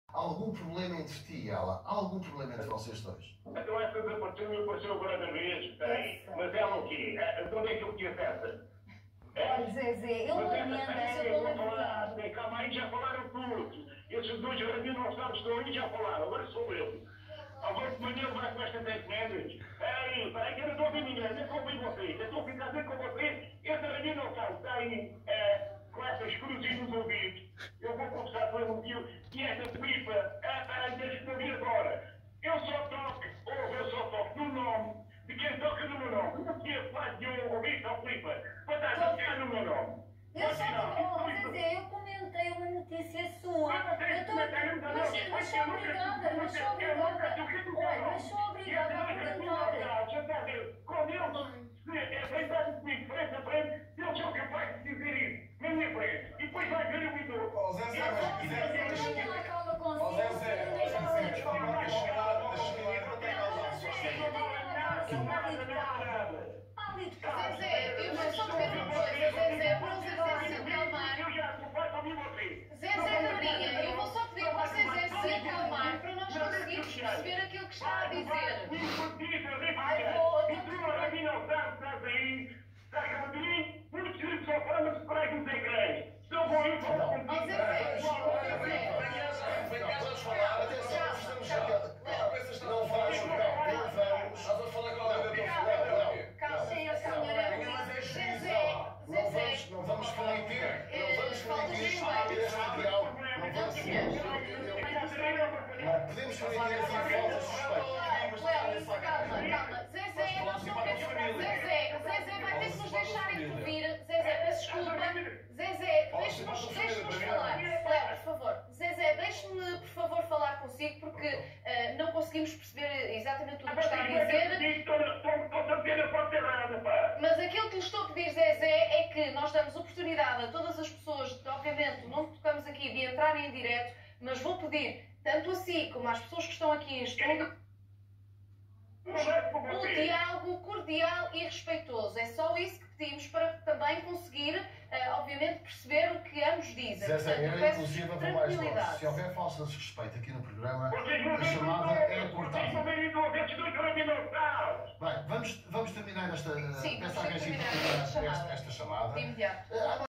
Zézé Camarinha na CMTV arma confusão com Maria Chuteira e Rabinossauros - imperdível!🤣🤣🤣
Zézé Camarinha ao seu melhor nível na CMTV